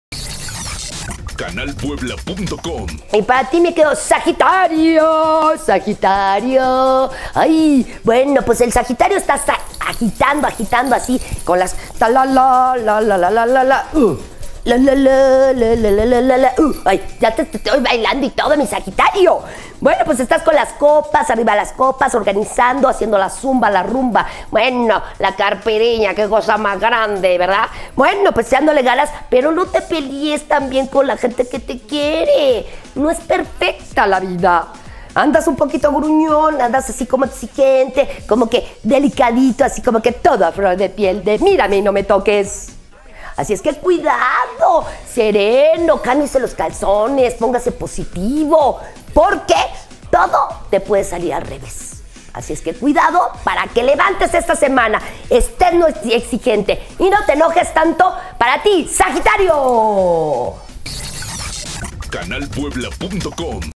Horóscopos con Claudia Contreras: SAGITARIO
Estás con las copas en las fiestas, no busques problemas con las personas. Cuídate.